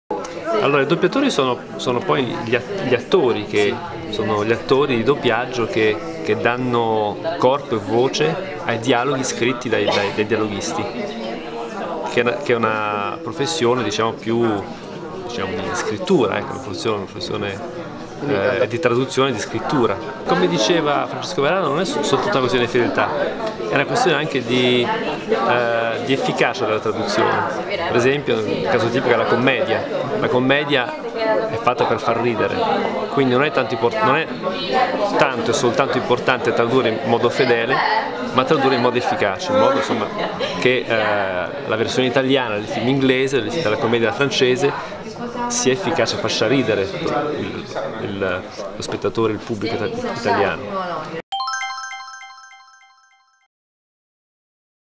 Capra Pictures: "Stefano Leoncini" Convegno Sala Sibilla 27ott. 2016, XVII Edizione
13Pillola_ Capra Pictures presenta: " Convegno alla Fortezza del Priamar Sala Sibilla , Stefano Leoncini" Diciassettesima edizione de Il Festival Nazionale del Doppiaggio Voci nell'Ombra, giovedì 27 ottobre 2016.